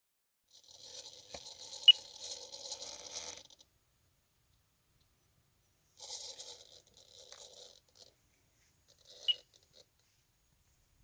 Freewing A-10 Twin 80 - Moving 'Nose-cam' pilot mod
Painless decap on the band saw for the stock pilot, servo up his back, some hot glue, a transmitter, lipo and an FPV cam for an oxygen mask.   All modular contained in the canopy, which can be swapped out for the stock one.

I don't plan on flying through this camera, but I do think it will be useful to prevent overturning and disorientation on final.  Plane is going to be hard to see and require a long approach.

Fun anyway...  Recorded vid from the 'Nose-cam' coming next.